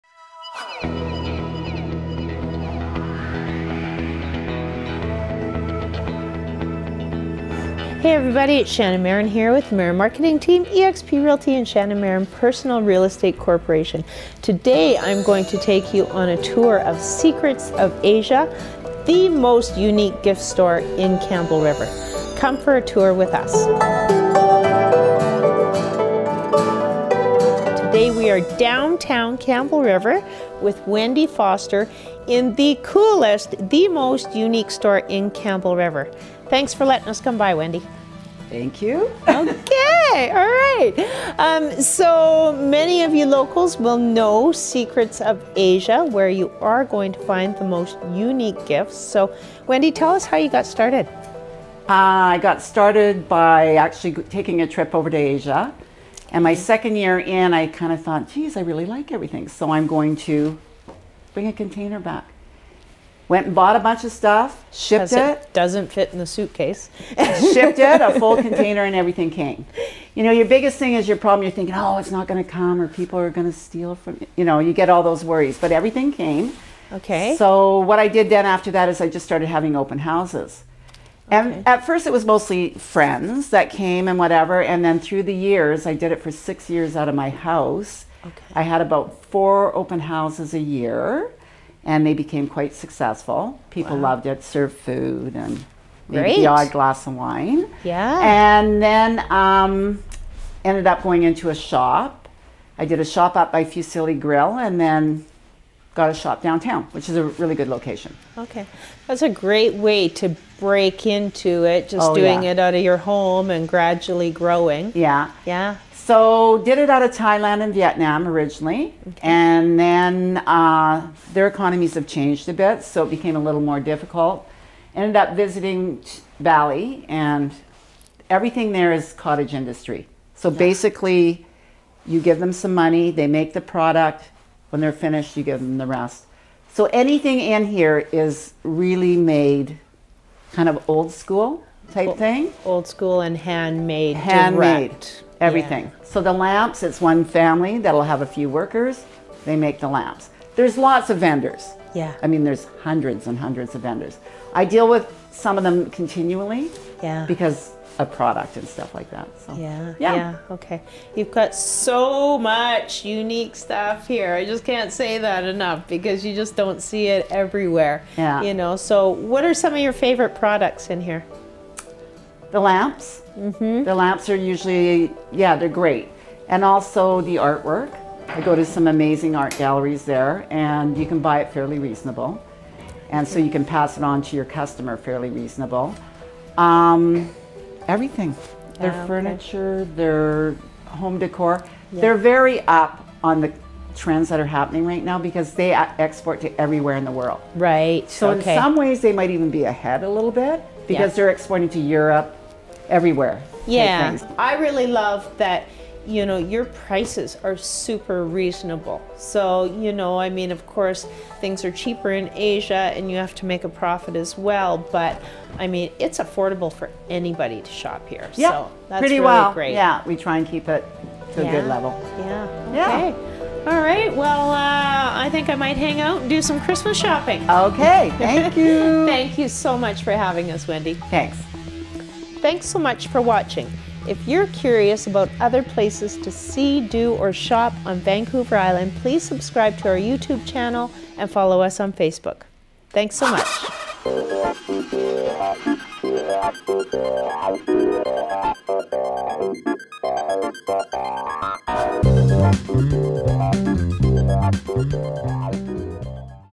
Secrets of Asia Imports
This week's Vancouver Island Vision brings us to Campbell River's Secrets of Asia Imports. You will find Secrets of Asia, and all of its unique belongings, in the heart of downtown Campbell River.

Wendy started Secrets of Asia after visiting Asia and loving their style of decor. She then bought a number of belongings and shipped them home.

Wendy started out by selling items out of her home for 6 years and found great success. Secrets of Asia's products are all handmade and unique to Asia.